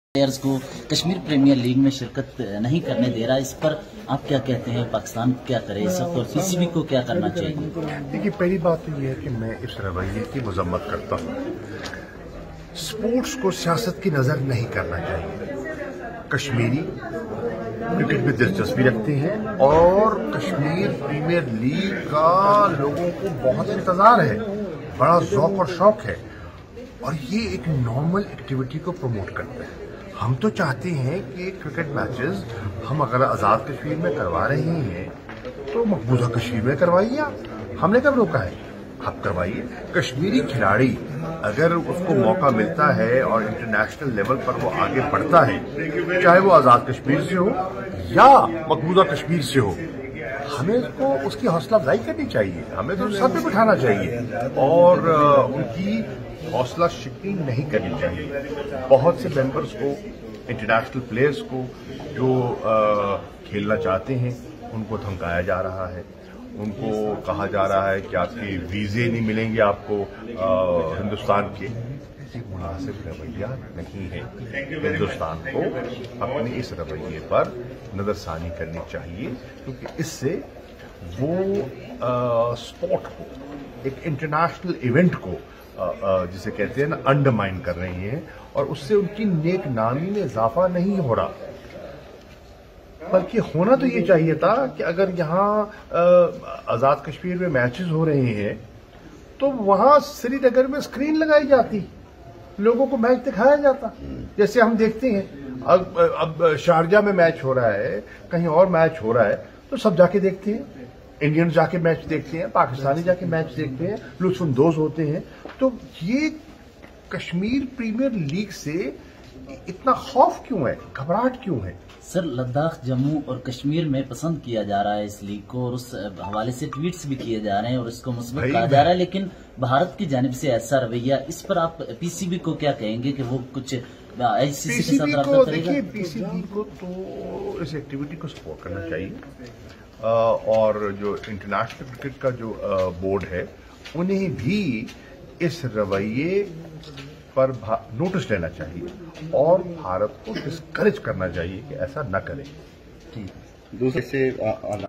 Minister of Foreign Affairs Shah Mahmood Qureshi Talks to Media at Parliament House in Islamabad
Minister of Foreign Affairs Shah Mahmood Qureshi Talks to Media at Parliament House in Islamabad (02.08.21)

وزیر خارجہ مخدوم شاہ محمود قریشی کی پارلیمنٹ ہاؤس میں میڈیا سے گفتگو

کشمیر پریمیر لیگ کا لوگوں کو انتظار ہے، مخدوم شاہ محمود قریشی

سپورٹس کو سیاست کی نذر نہیں کرنا چاہیے، شاہ محمود قریشی

میں بھارت کے رویے کی مذمت کرتا ہوں، شاہ محمود قریشی

کشمیر پریمیر لیگ جیسے ایونٹ نارمل ایکٹیویٹی کو پرموٹ کرتے ہیں، مخدوم شاہ محمود قریشی

ہم پریمیر لیگ جیسے ایونٹ مقبوضہ کشمیر میں کروائیے، آپ کو کس نے روکا ہے، شاہ محمود قریشی

کشمیری کھلاڑی خواہ وہ آزاد کشمیر سے ہو یا مقبوضہ کشمیر سے، اگر آگے بڑھتا ہے تو ہمیں اس کی حوصلہ افزائی کرنی چاہیے، مخدوم شاہ محمود قریشی

بھارت کے رویے سے ان کی نیک نامی میں اضافہ نہیں ہو رہا، شاہ محمود قریشی

کھلاڑیوں کو دھمکایا جا رہا ہے کہ انہیں ویزے نہیں ملیں گے، مخدوم شاہ محمود قریشی

چاہیے تو یہ تھا کہ آزاد کشمیر میں ہونیوالے میچز کو سری نگر میں سکرین لگا کر دکھایا جاتا، جیسے شارجہ میں دکھایا جاتا ہے مخدوم شاہ محمود قریشی

انہیں کشمیر پریمیر لیگ کا اتنا خوف کیوں ہے؟

پاکستان کرکٹ بورڈ کو اس ایکٹیویٹی کو سپورٹ کرنا چاہیے، شاہ محمود قریشی

انٹرنیشنل کرکٹ بورڈ کو بھی بھارت کے اس رویے کا نوٹس لینا چاہیے، مخدوم شاہ محمود قریشی

Foreign Minister Makhdoom Shah Mahmood Qureshi Monday while condemning Indian negative attitude over Kashmir Premier League (KPL) called upon the International Cricket Council (ICC) to take notice of Indian response.

Talking to media in the Parliament House, the foreign minister said that KPL like events promoted the normal activity and sports should not be politicized.

He said that people were eagerly waiting for the start of much exciting cricket event– Kashmir Premier League (KPL).

“The Kashmiri player whether belonged to Azad Kashmir or IIOJK, he should be encouraged to excel,” Qureshi said calling upon the Indian government to conduct the same event in IIOJK.

He further said that Indian response to the KPL was not endorsed as it was coercing the aspiring cricketers to quit with threats of refusal of grant of visas.

The foreign minister said that the KPL event must be streamed on Tv screens in Srinagar.